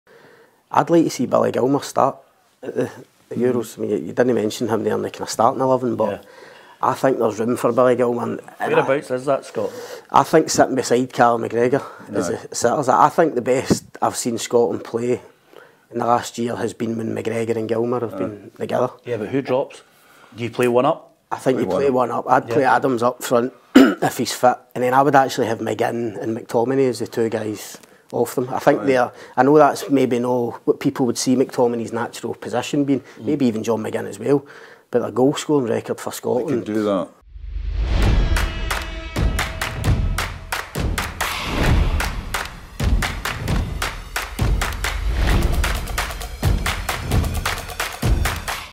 'Billy Gilmour should be a STARTER at Euro 2024' - but who drops out of Scotland XI?
🗣️'I'd like to see Billy Gilmour start...I think there's room for him in the starting XI'

Daily Record & Sunday Mail journalist
believes that the Brighton midfielder should be a starter at Euro2024